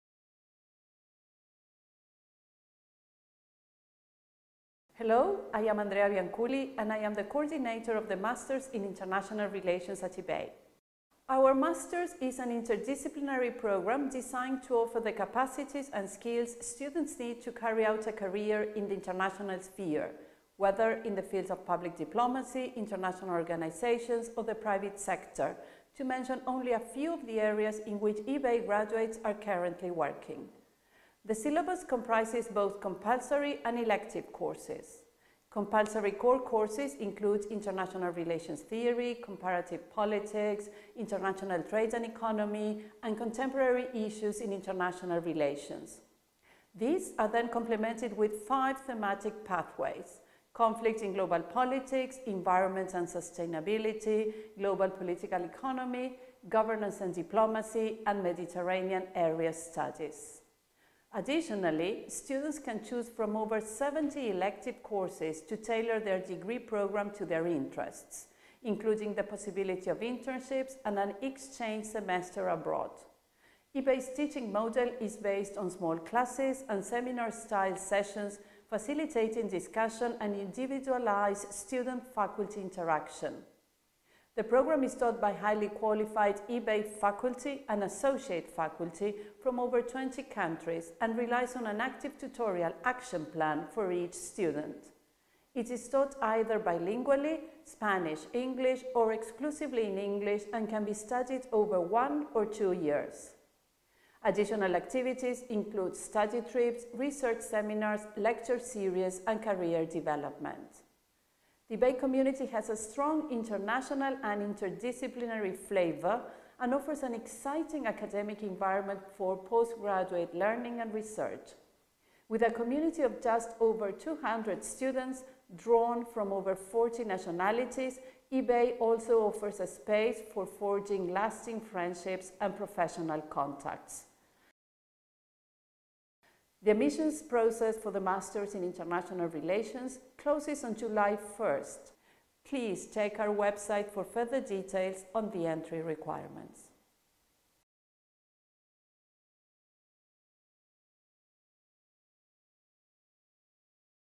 Master's in International Relations
Since its beginning in 2004, the Master's in International Relations, has consolidated itself, both in Spain and abroad, as a leading programme in the field of international affairs. It offers a solid academic preparation for students intending to pursue a professional career in any of the social, political and economic domains of international relations.